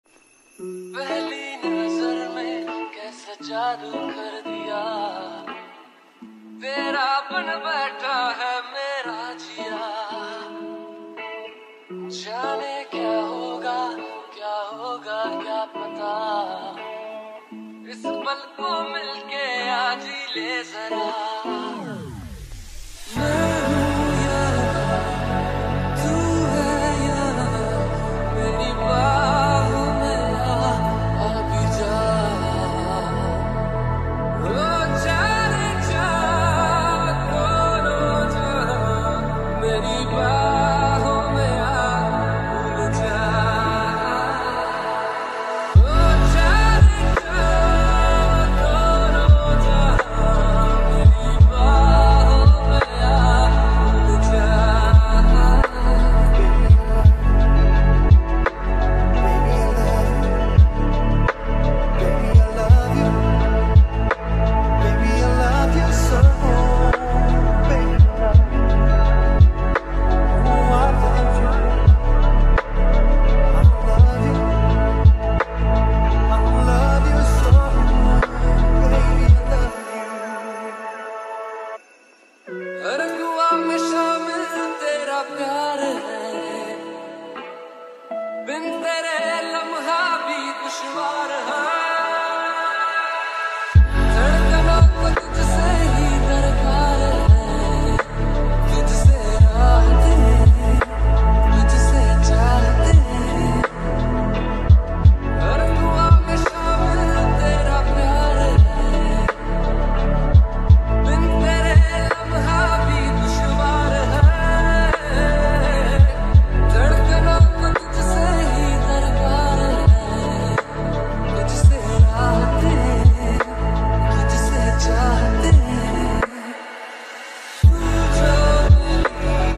Making of homemade kasuri methi mathi🤎
This video is about making of kasuri methi at home,